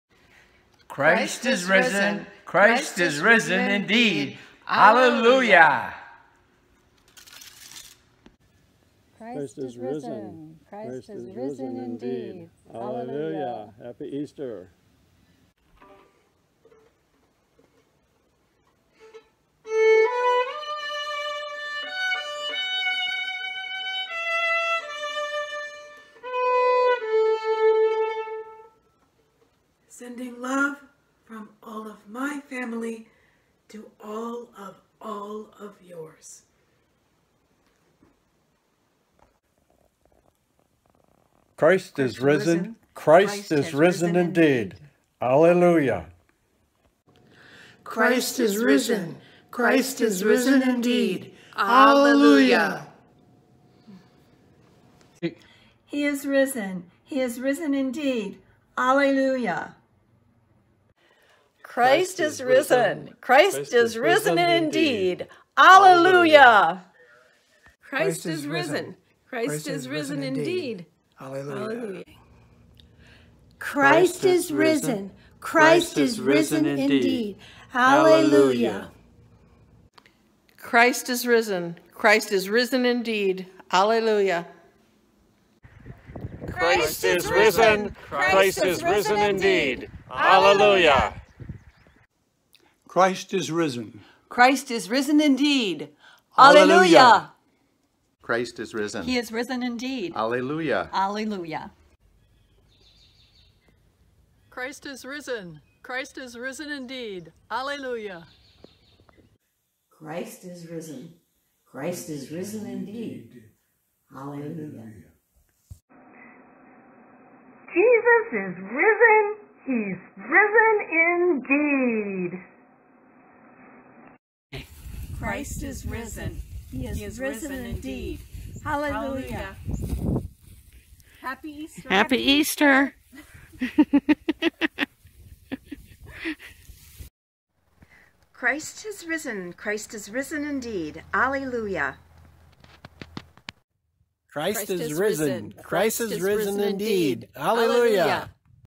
Christ Is Risen Greetings
Members of the Shepherd of the Bay community share their Easter greetings!